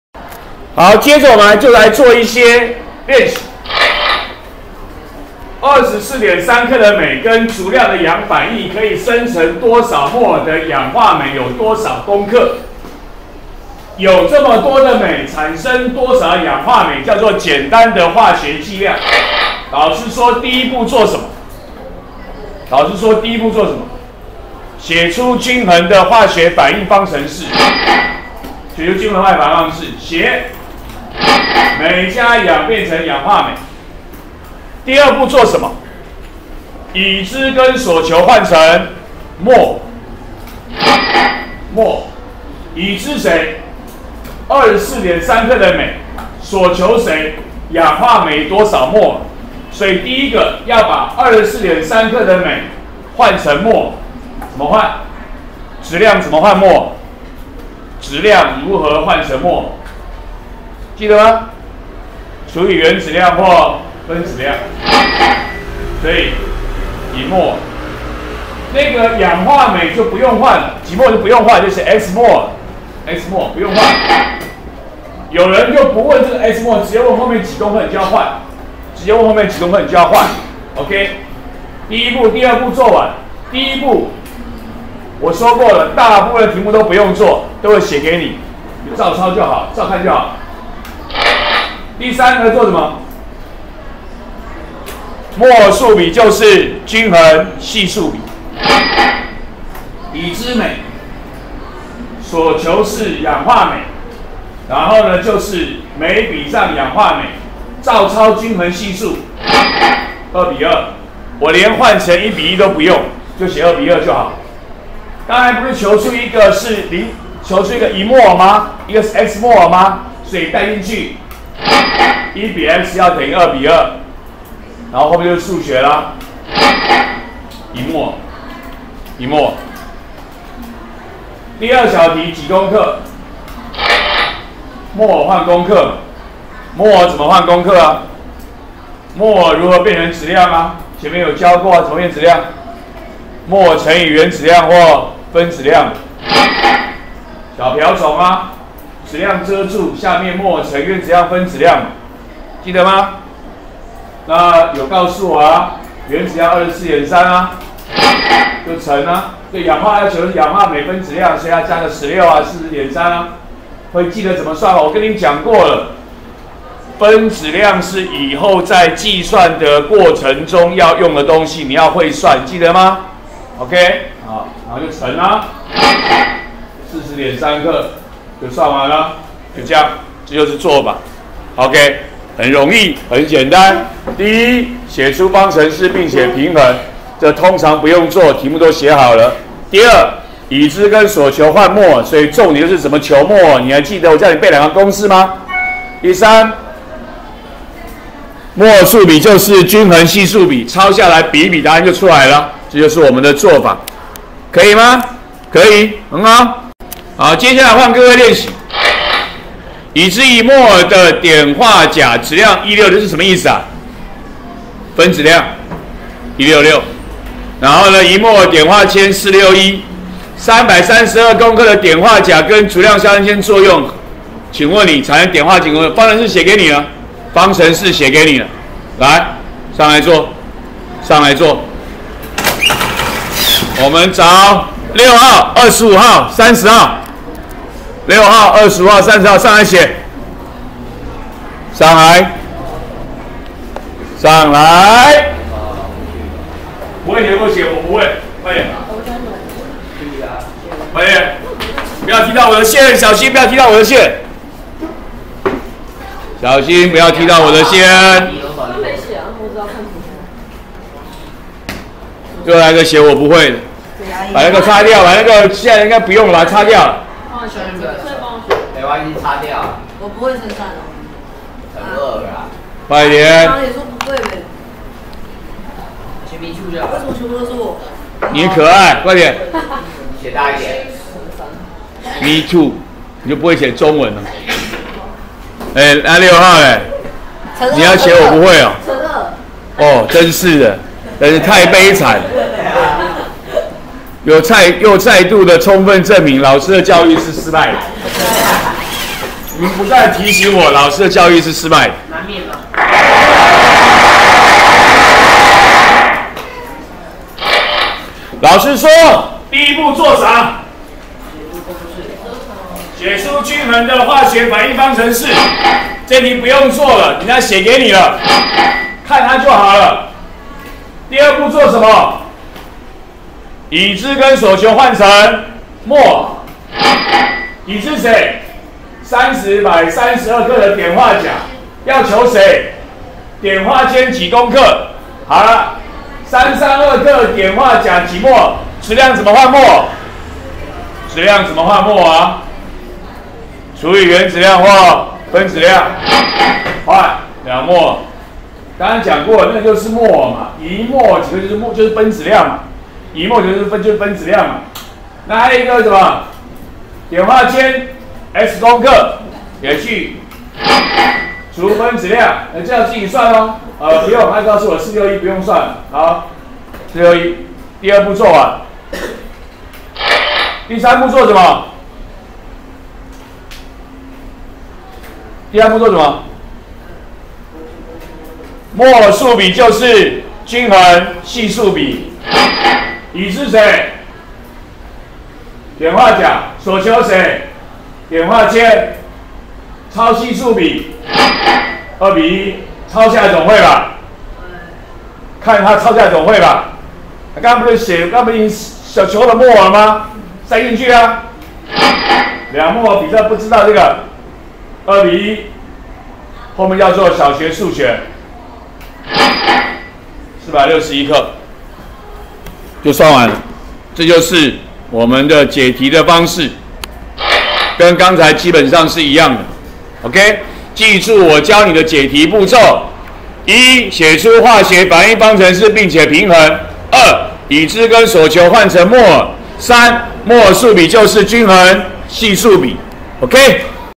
103學年國二理化 化學反應16：例題04、練習05
例題04是24.3克鎂和氧作用，求產生氧化鎂多少莫耳、多少公克。練習05是332克碘化鉀和足量硝酸鉛作用，問產生多少克碘化鉛。